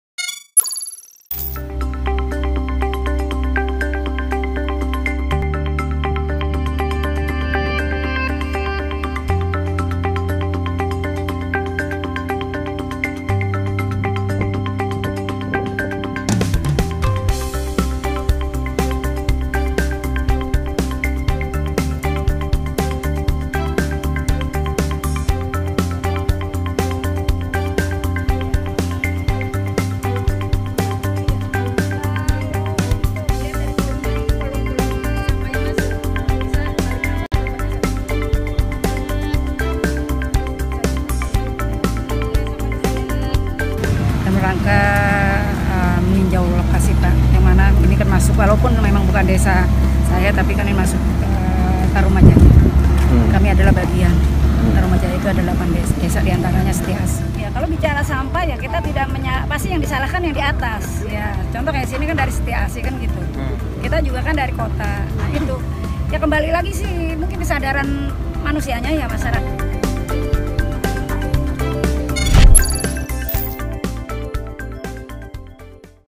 Gaya Nyentrik Ibu Lurah Tinjau Kali Sampah Pisang Batu
Ibu Lurah Desa Setia Asih, Siti Qomariyah meninjau Kali Pisang Batu yang berada di Desa Pahlawan Setia, Tarumajaya, Kabupaten Bekasi. Tampak Ibu Lurah memakai kacamata hingga dipayungin umbrella girl saat meninjau lokasi. Duh!